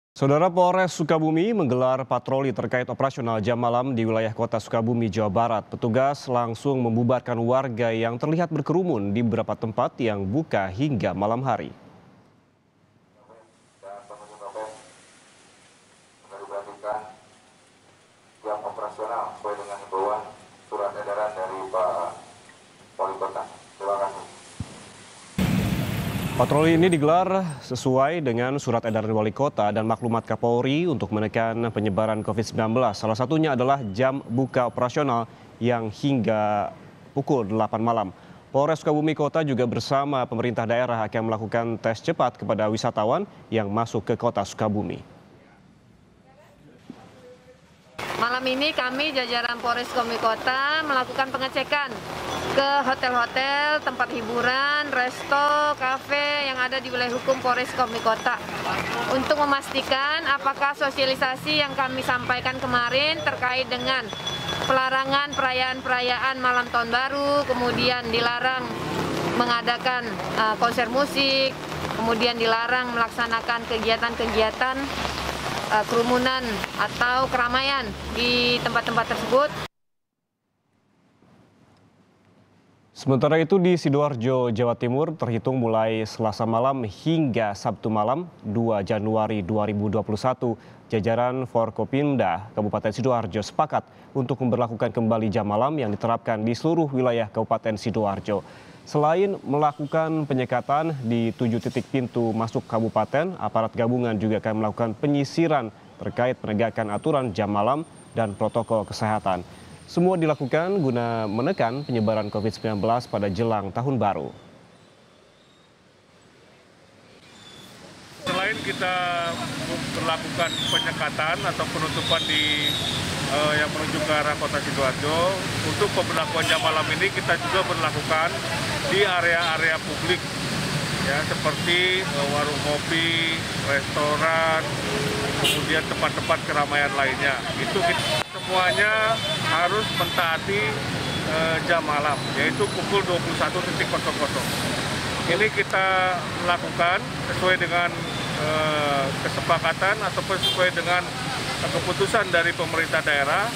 Polisi Gelar Patroli Malam dan Bubarkan Kerumunan di Kota Sukabumi
SUKABUMI, KOMPAS.TV - Polres Sukabumi menggelar patroli terkait operasional jam malam di wilayah Kota Sukabumi, petugas langsung membubarkan warga yang terlihat berkerumun di beberapa tempat yang buka hingga malam hari.

Patroli ini digelar sesuai dengan surat edaran walikota dan maklumat Kapolri, untuk menekan penyebaran covid-19, salah satunya adalah jam buka operasional yang hingga pukul 8 malam.

Polres Sukabumi kota juga bersama pemerintah daerah akan melakukan tes cepat kepada wisatawan yang masuk ke Kota Sukabumi.

Sementara itu di Sidoarjo Jawa Timur, terhitung mulai Selasa malam, hingga Sabtu malam, 2 Januari 2021.

Jajaran Forkopimda Kabupaten Sidoarjo, sepakat untuk memberlakuan kembali jam malam, yang diterapkan di seluruh wilayah Kabupaten Sidoarjo.

Selain melakukan penyekatan, di 7 titik pintu masuk kabupaten, aparat gabungan juga akan melakukan penyisiran terkait penegakkan aturan jam malam, dan protokol kesehatah.

Semua dilakukan guna menekan penyebaran covid 19, pada jelang tahun baru.